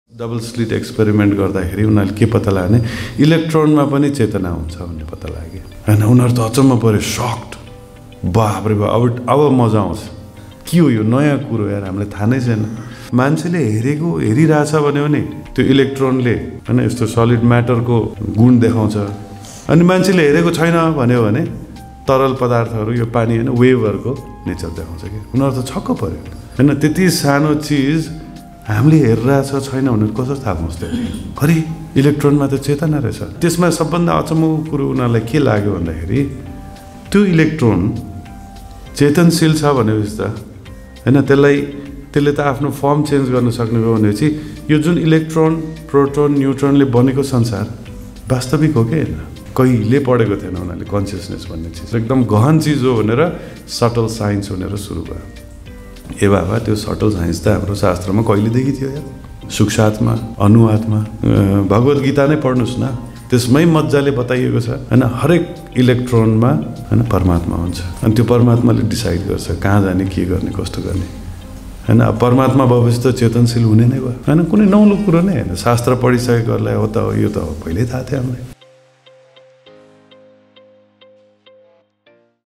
Scientist Shock मा-Electron मा पनि चेतना हुन्छ | Young's Double Slit Experiment | Rupeshwor Gaur Das
Electron have consciousness, electron consciousness, matter, subtle science